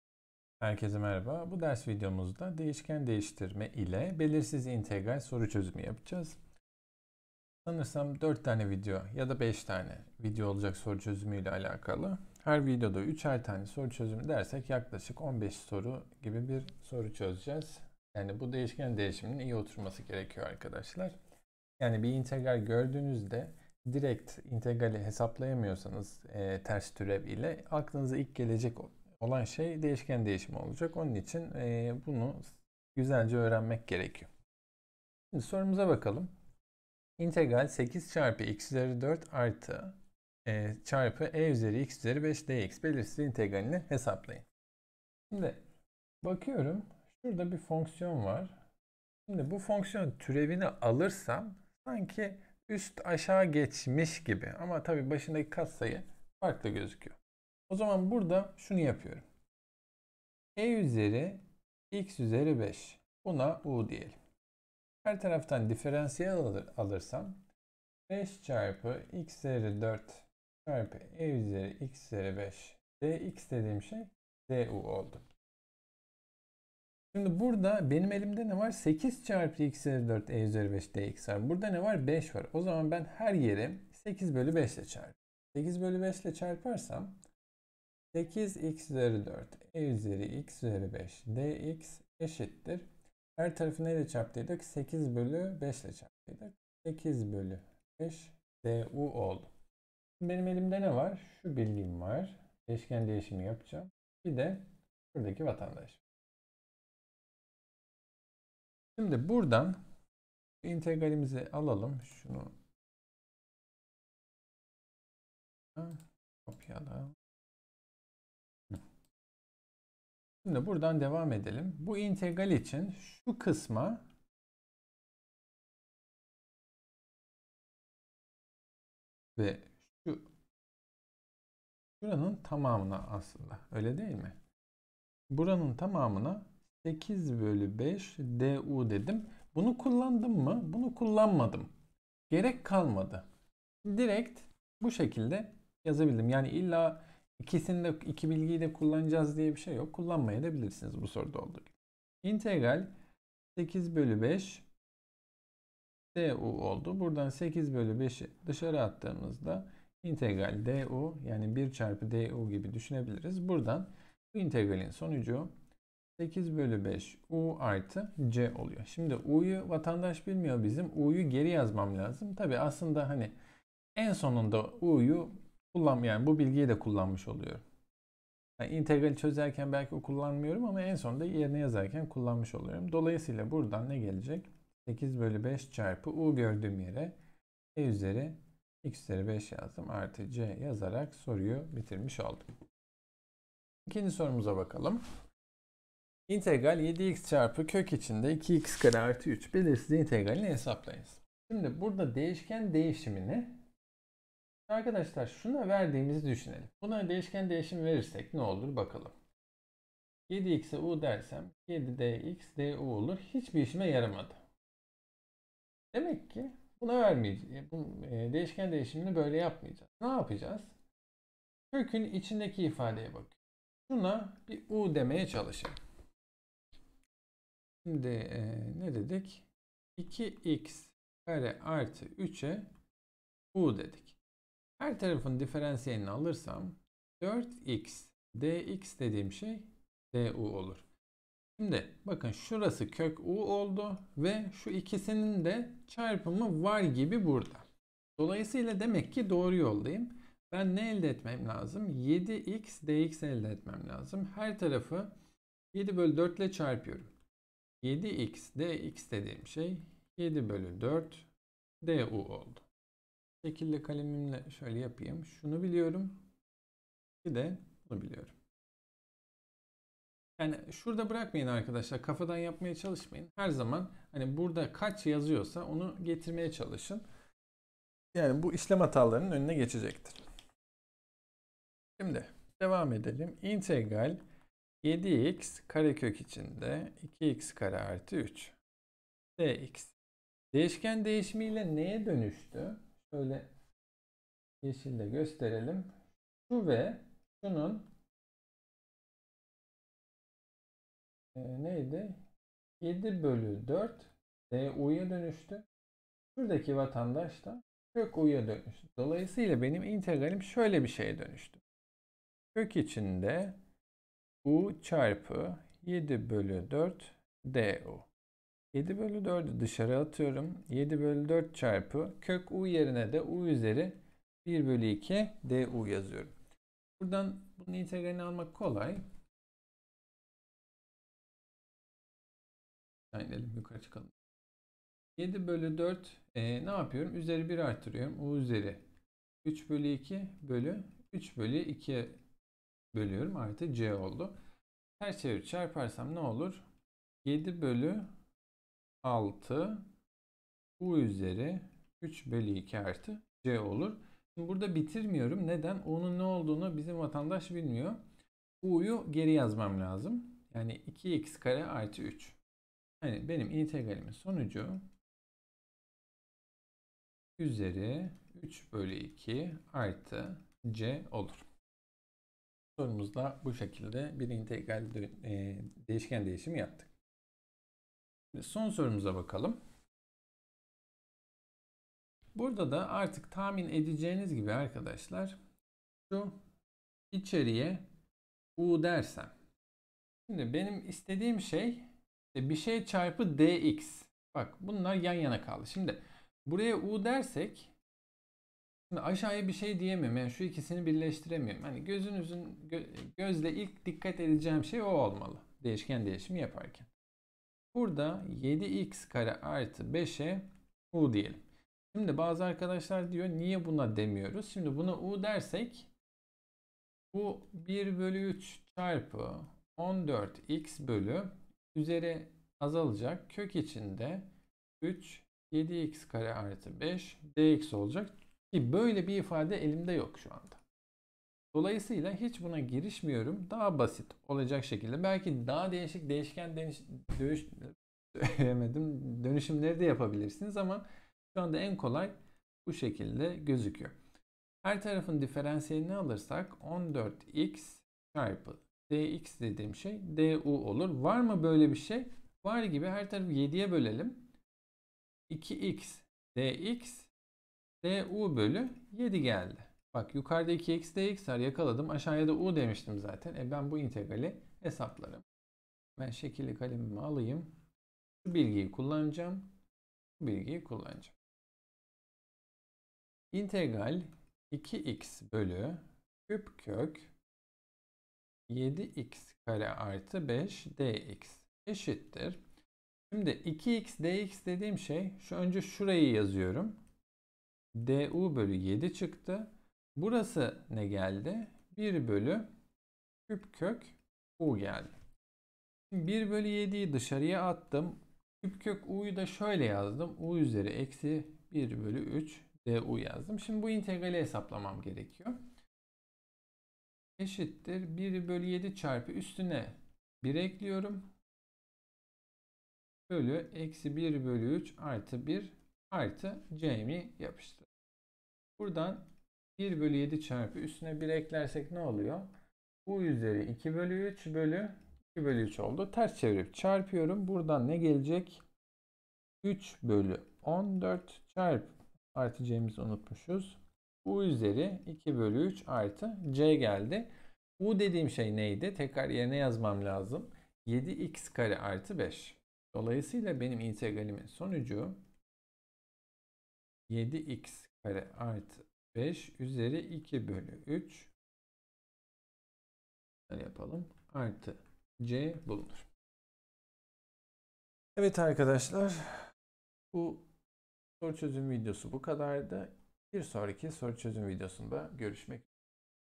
5) Belirsiz İntegralde Değişken Değişimi Soru Çözümü I
Bu videoda değişken değişimi ile belirsiz integral hesaplama ile ilgili 3 örnek soru çözülmüştür.

Matematiği seven insanlar olarak her şeyi konuşabilmek için kurduğum bu kanalda çeşitli alt alanlarda detaylı videolar bulabilirsiniz.